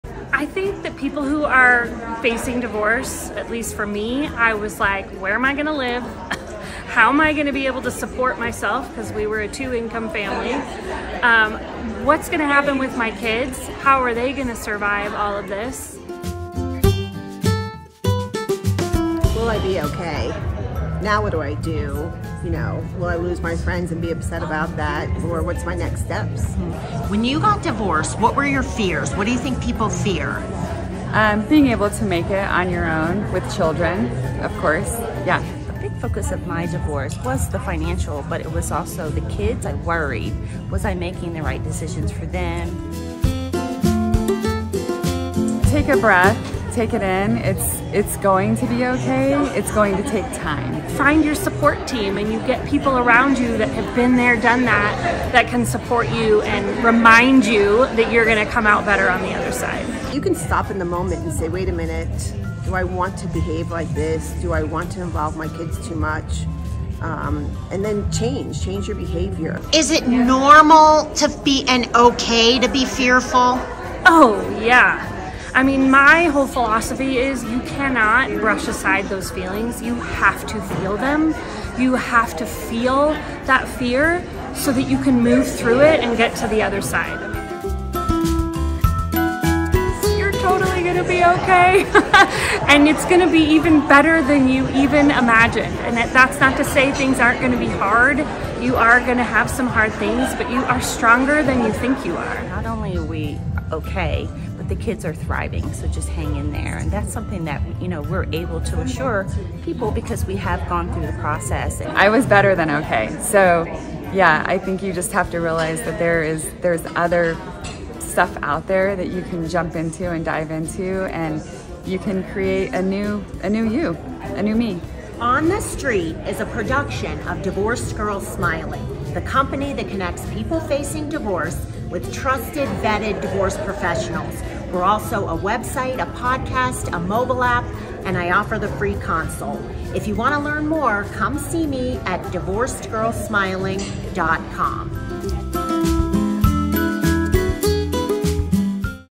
On the street #3: Fear During Divorce
It's normal to have a lot of fear during divorce. So how do you cope? I asked a few divorce professionals, all who have been through a divorce and faced their fears.